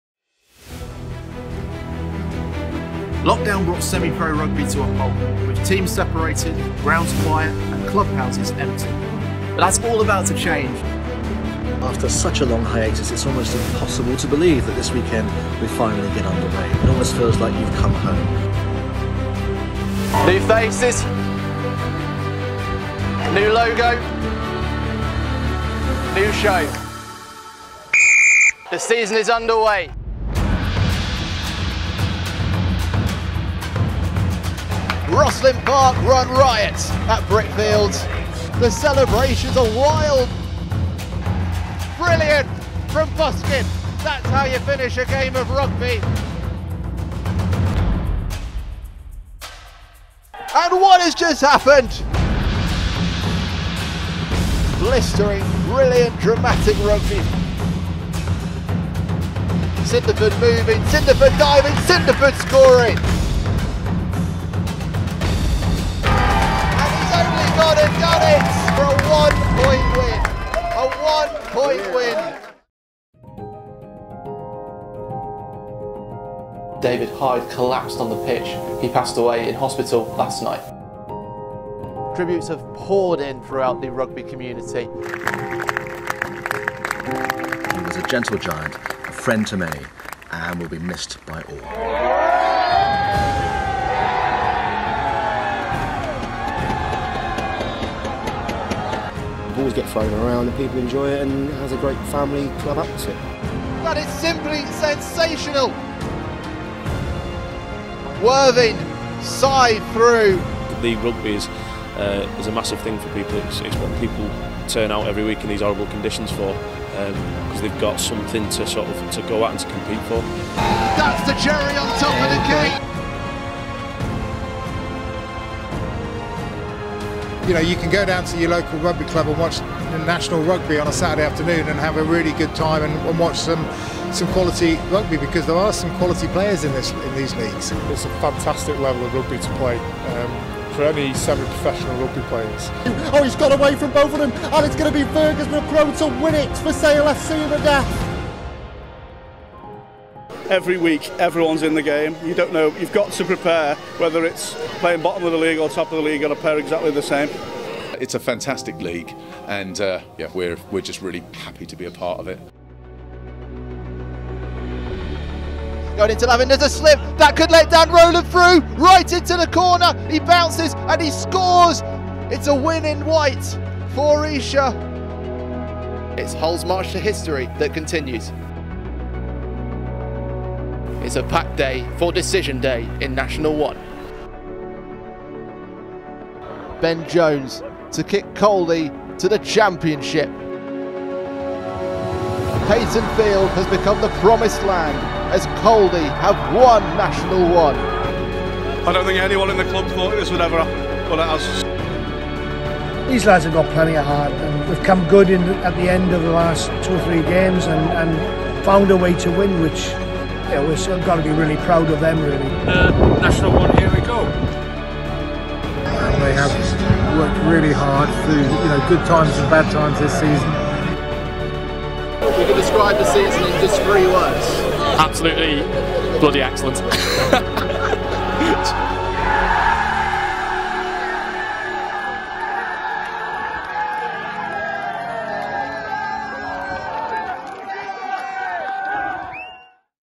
The National League Rugby Review Show | 21/22 End Of Season Montage
The National League Rugby Review Show covered every moment of a sensational 2021/22 season in National 1, National 2 North and National 2 South!

This montage thanks those who contributed to the Show's production throughout the season while reflecting on the best moments from the season, including Caldy, Esher and Hull's promotions.

What was your favourite National League Rugby moment from the 2021/22 season?

Originally aired 2nd May 2022.

Click the 🔔 & be the first to see our videos!